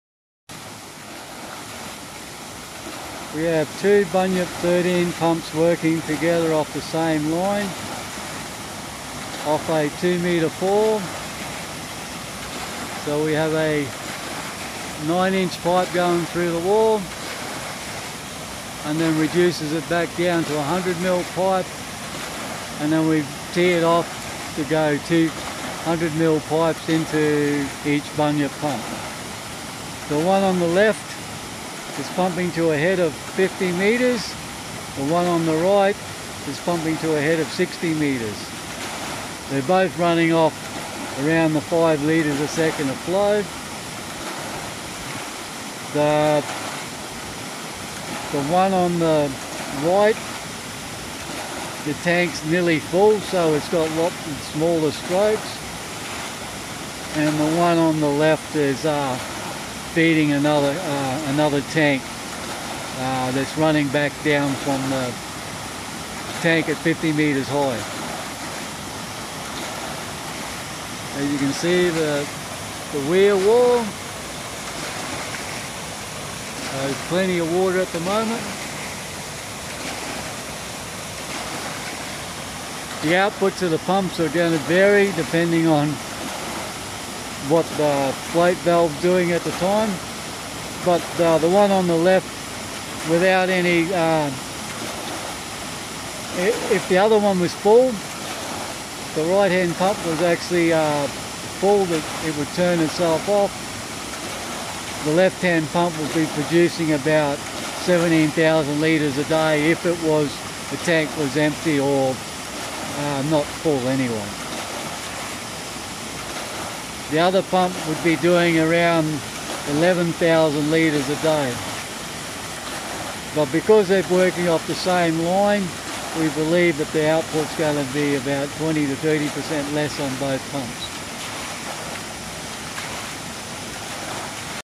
Two Bunyip Pumps working off the same supply pipe.
Two Bunyip PA 13 Pumps working off the same supply pipe. the Fall is 2 metres and one on the left is pumping to a 50 metre head and the other is pumping to a 60 metre head. Suppling water to a dairy herd off 300.